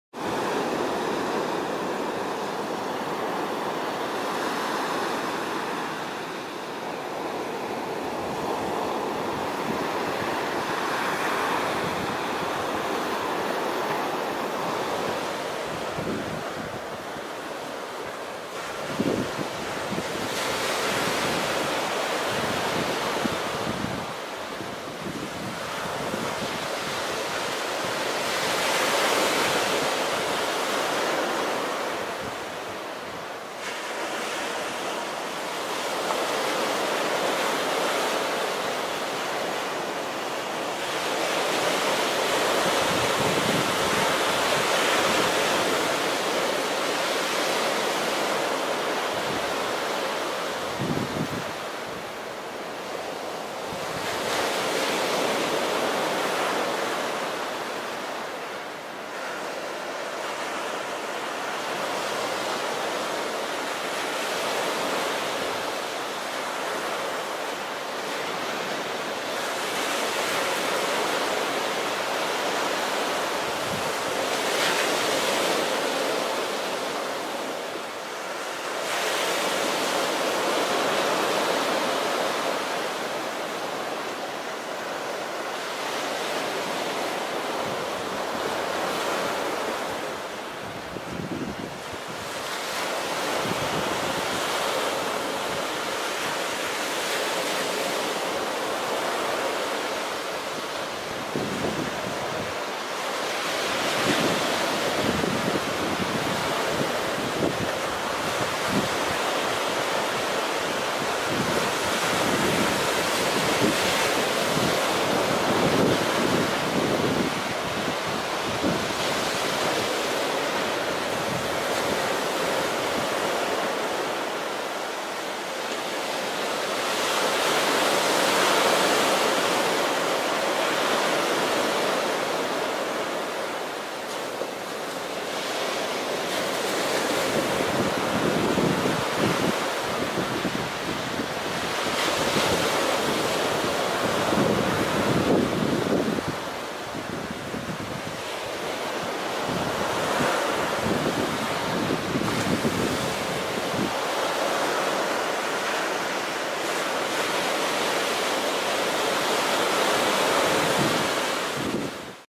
Побережье Кассандры (Халкидики) перед рассветом
Побережье Кассандры (Халкидики) перед рассветом, пляж отеля Менди